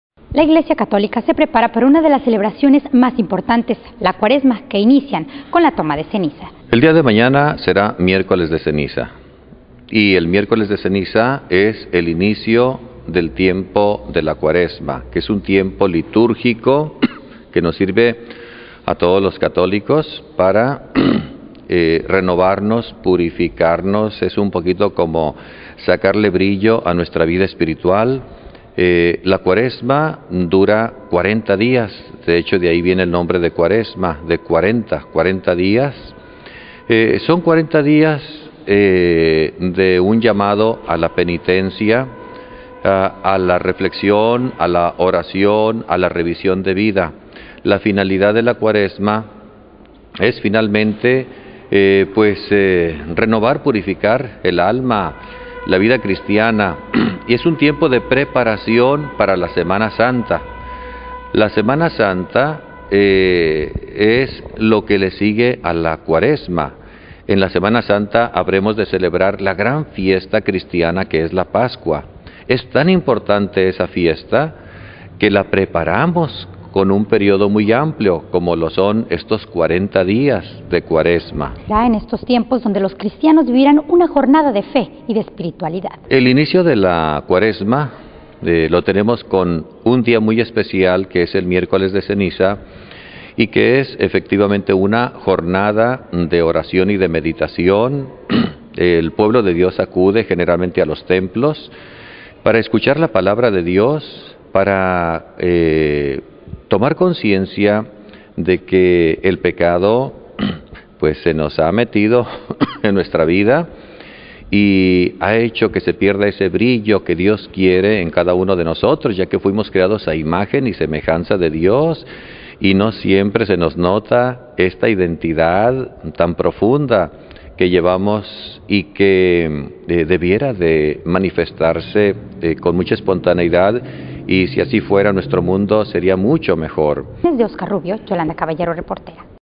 Llega la Pascua a Rosarito
La cuaresma inicia hoy miércoles, y la iglesia católica se prepara para recibir a miles de feligreses. Se esperan miles de seguidores para acudir a la unción de la ceniza.
Este es un momento de flexión y oración, puntualizaron católicos.